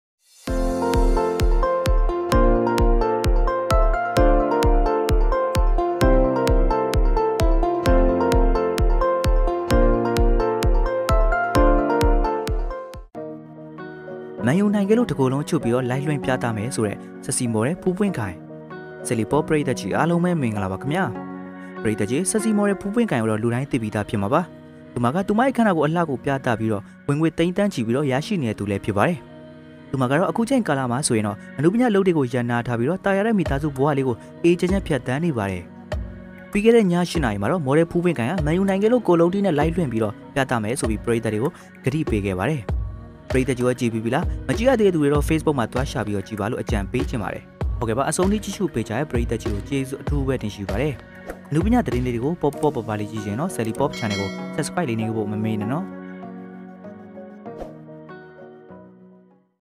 မန်ယူနိုင်လို့တစ်ကိုယ်လုံးချွတ်ပြီးLiveလွှင့်ပြသခဲ့တဲ့"ဖူးပွင့်ခိုင်"ဗီဒီယို
cele star
cele new
myanmar new
myanmar model
myanmar movie
myanmar cele